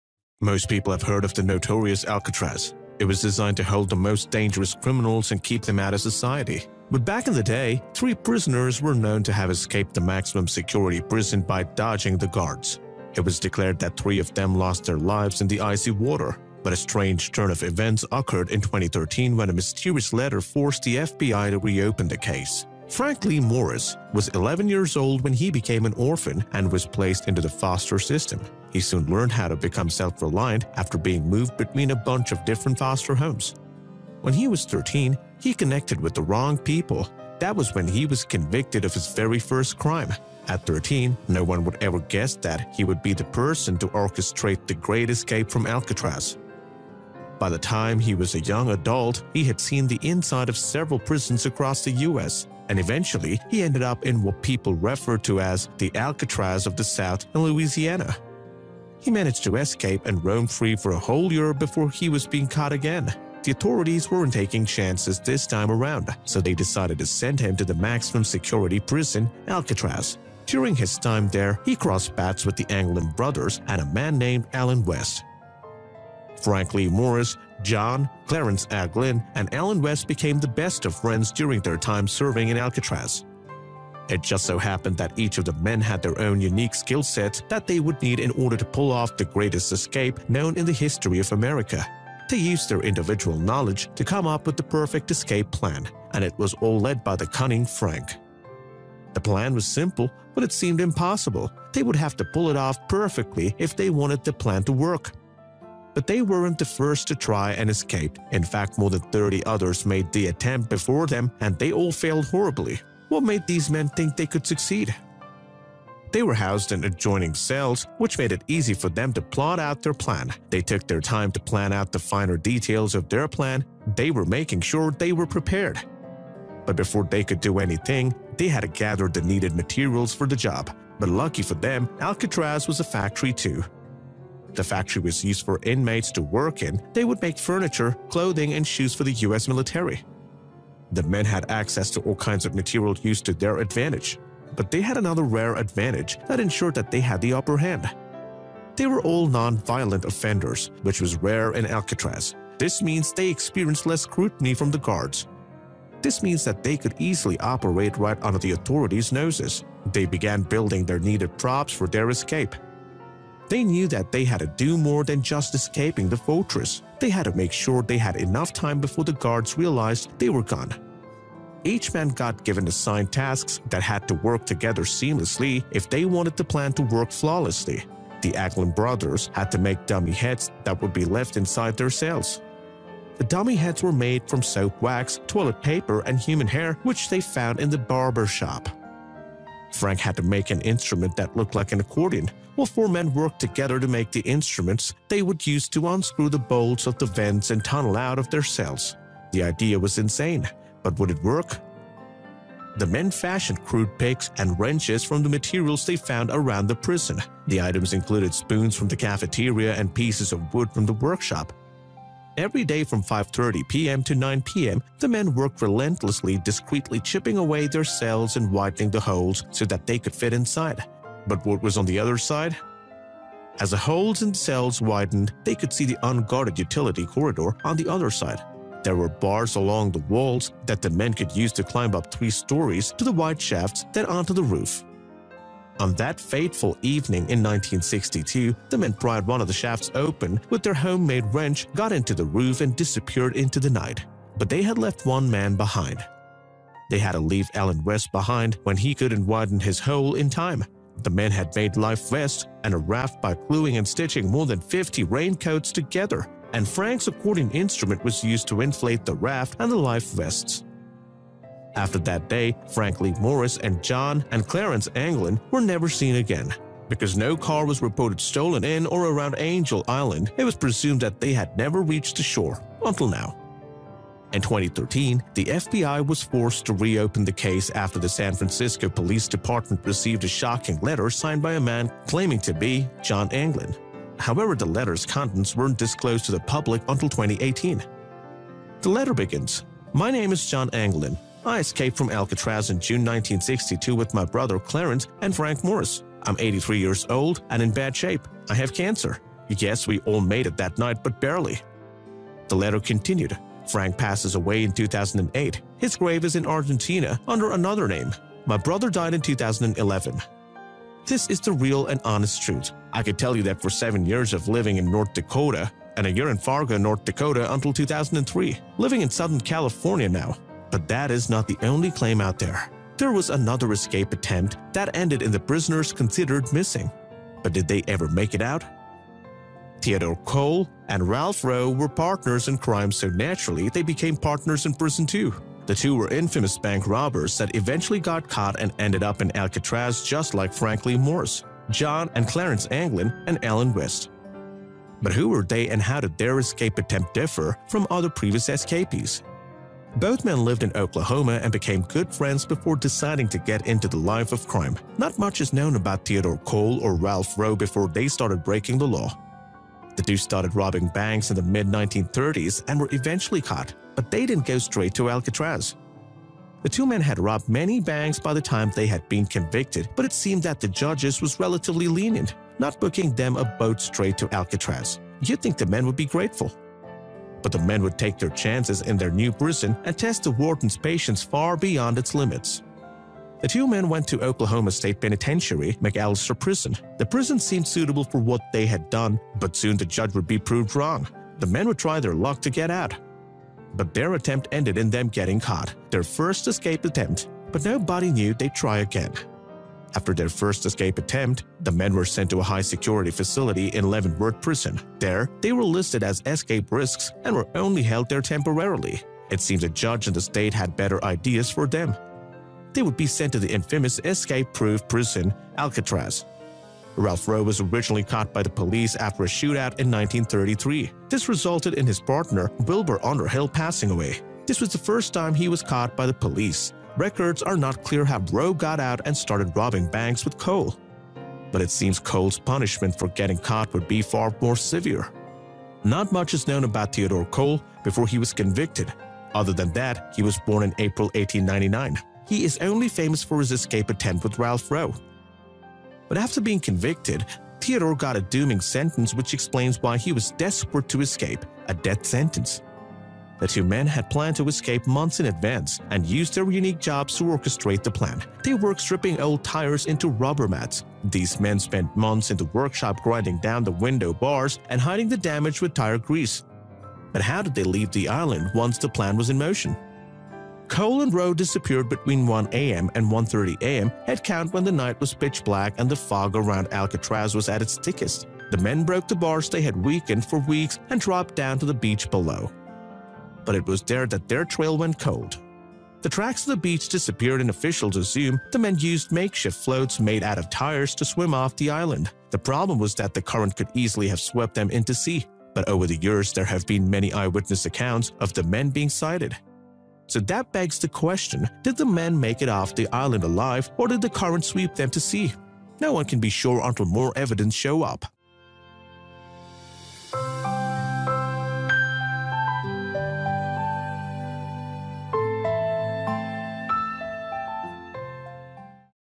55 Years Later - Man Who Escaped Alcatraz Sends FBI This Note
55 Years Later - Man Who Escaped Alcatraz Sends FBI This Note

 or